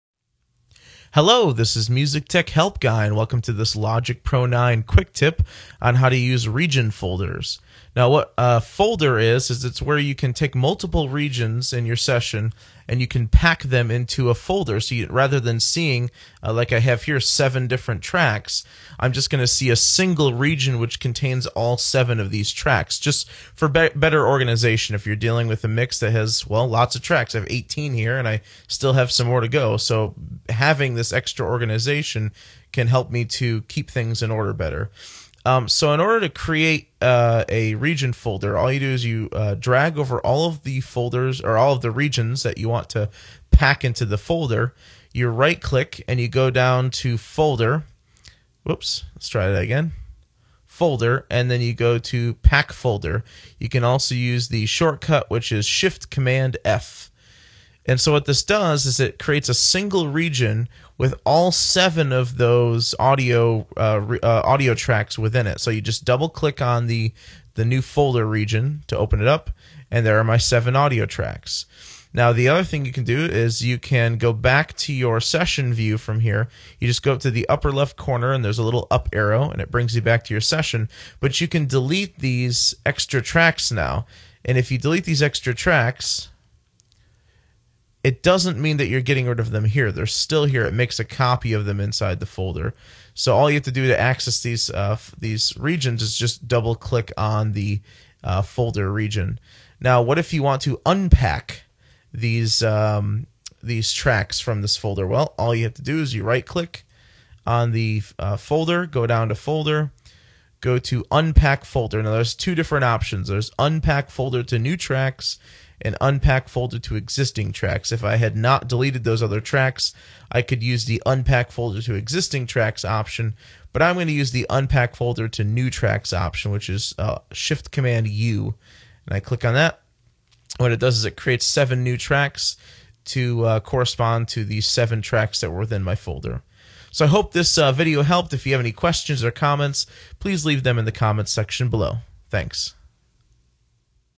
Logic Pro 9 Quick Tip - How to Pack Regions into Folders
A short video demonstrating how to pack regions into folders for better organization within your session.